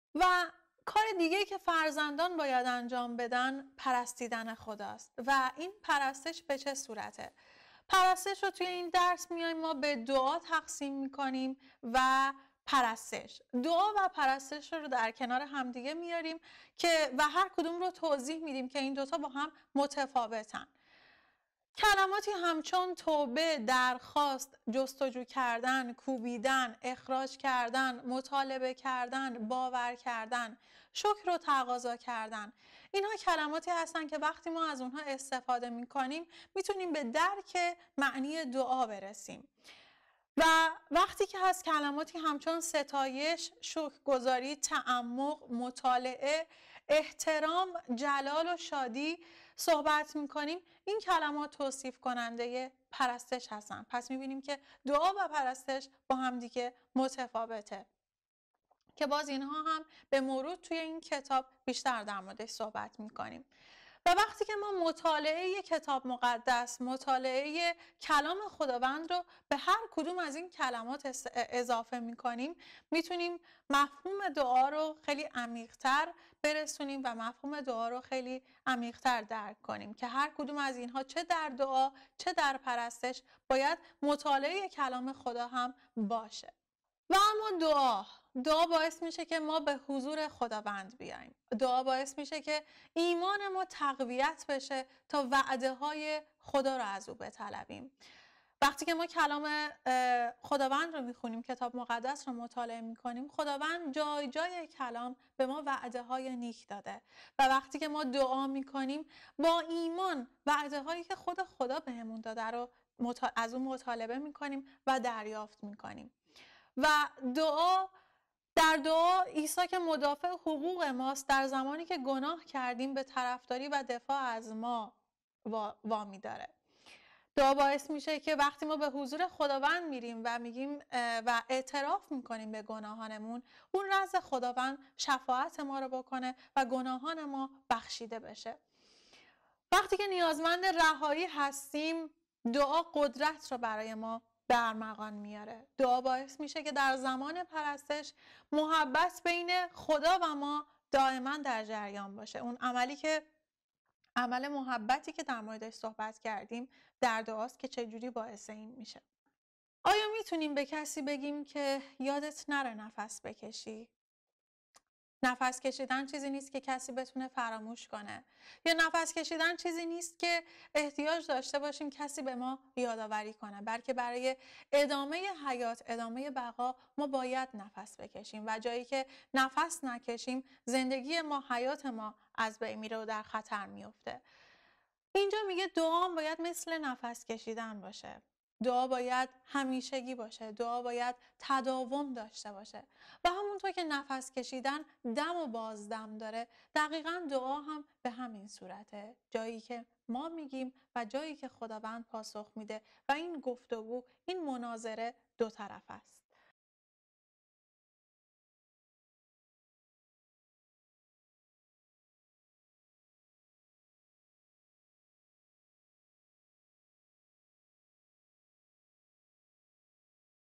توصیف واژه های دعا و پرستش - تعالیم مسیحیت
تفاوت بین دعا و پرستش چیست ؟
تعالیم کتاب مقدس و مسیحیت
کارگردان و مجری :
الهام عربی
میلاد ابراهیم پور
فیلمبردار و مدیر صحنه :
سامان عزیزی
کلیسای فارسی زبانان هماربی - استکهلم
منابع:
کتب دوره های خدمات مسیحی از انتشارات ایلام
دیگر کتب معتبر مسیحی
Directors:
Milad Ebrahimpour
Elham Arabi
Hammarbykyrkan EFS
Nathorstvägen 44, 121 37 Johanneshov